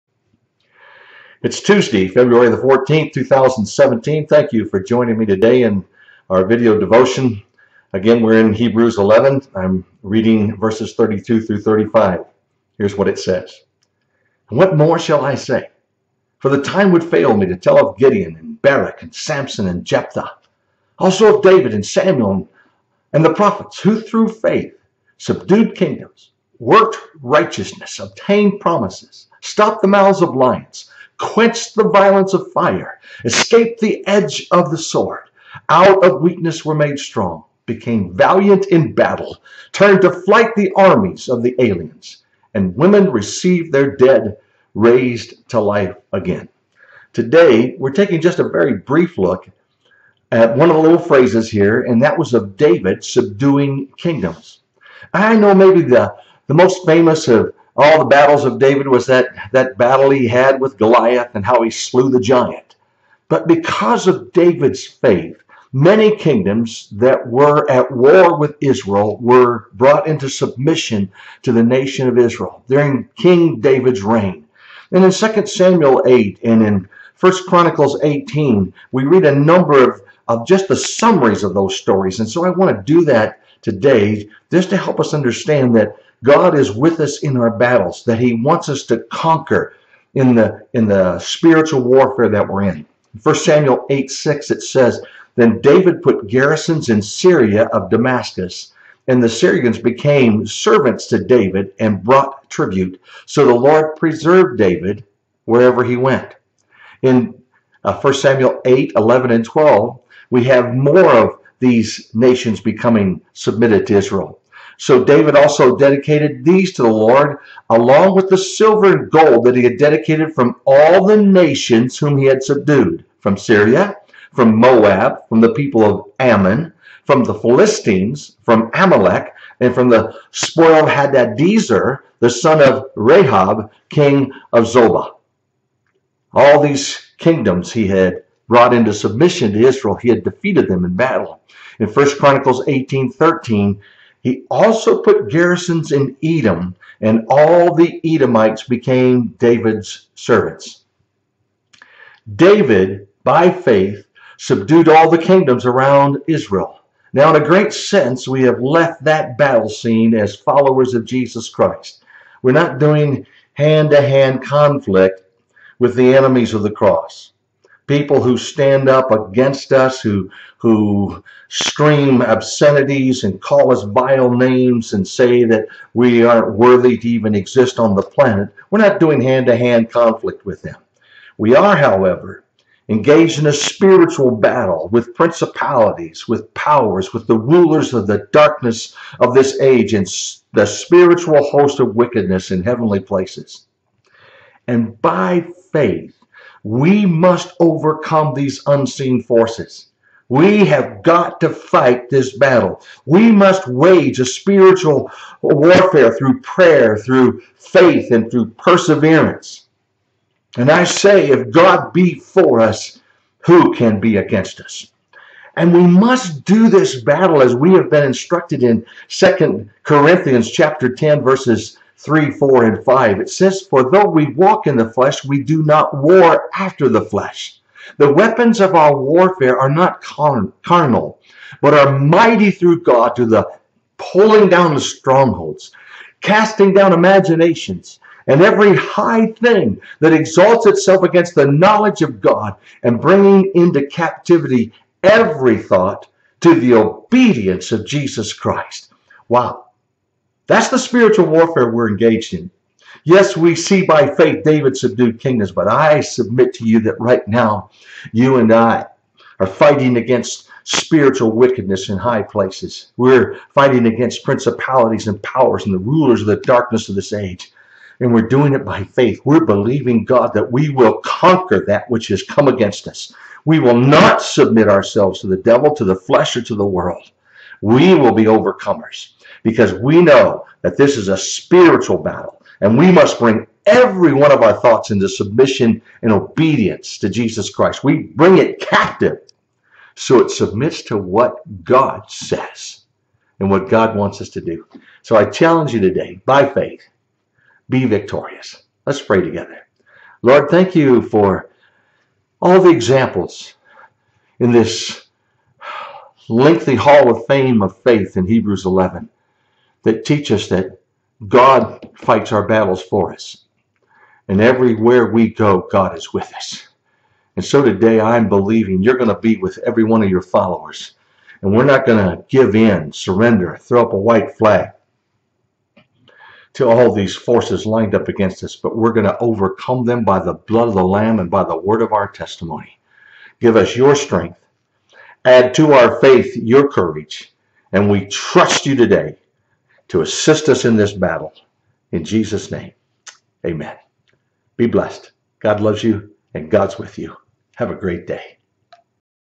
Tuesday February 14, 2017
Hebrews 11:32-35 Taking a look at David subduing kingdoms.  I Samuel 8:6; 11-12, I Chronicles 18:13 and 2 Corinthians 10:3-5  where we learn how to fight spiritual warfare.